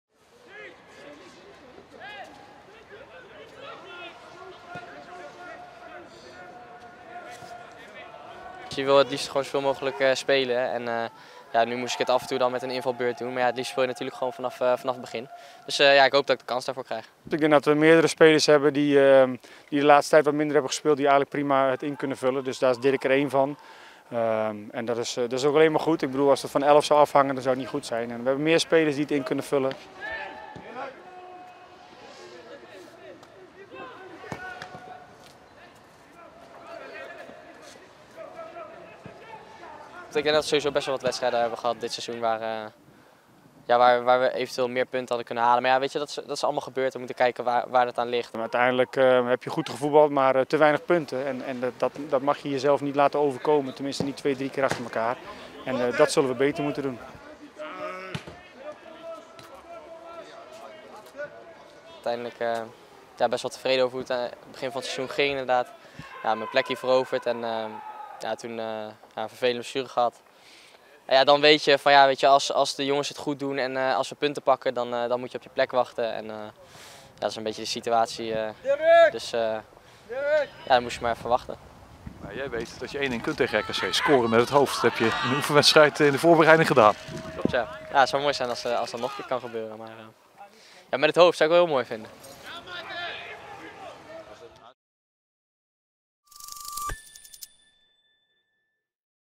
Dirk Proper terug in de basis bij NEC
Nijmegen - Dirk Proper was bezig aan een prima seizoen bij NEC totdat hij geblesseerd raakte. En toen hij weer fit was, kwam hij op de reservebank terecht.